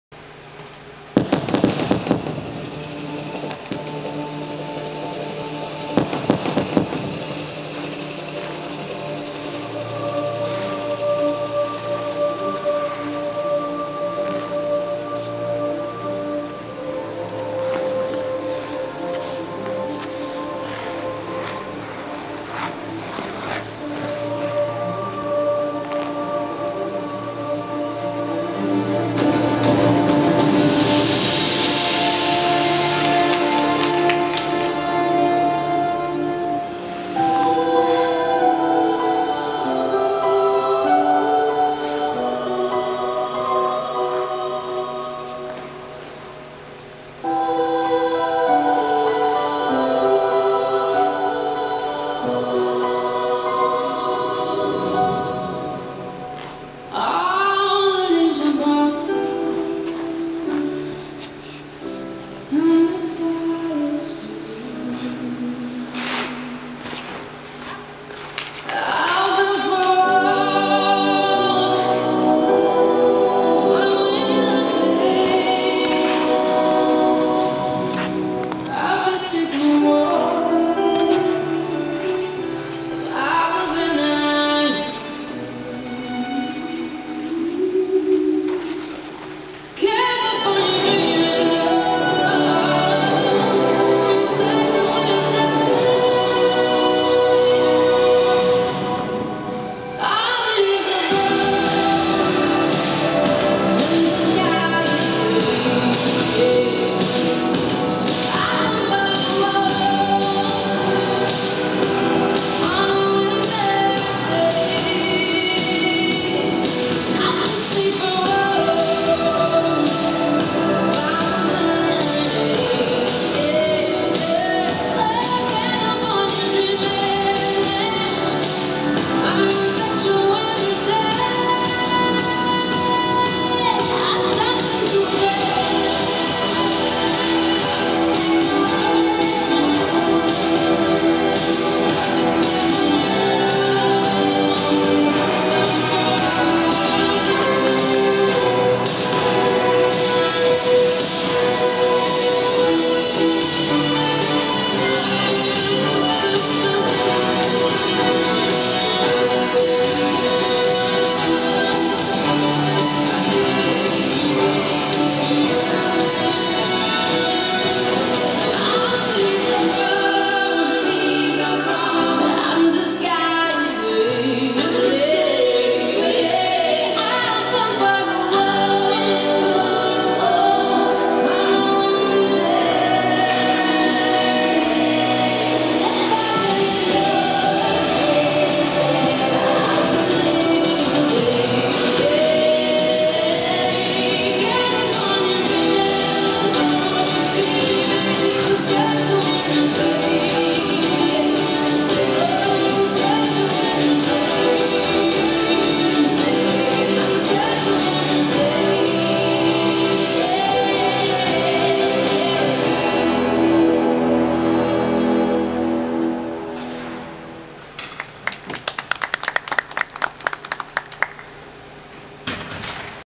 6급 FS 01 배민지,대구정화중1 @ 2015년 4차 피겨 승급심사
2015/12/14, 태릉 실내빙상장, 2015년 4차 5-8급 피겨승급심사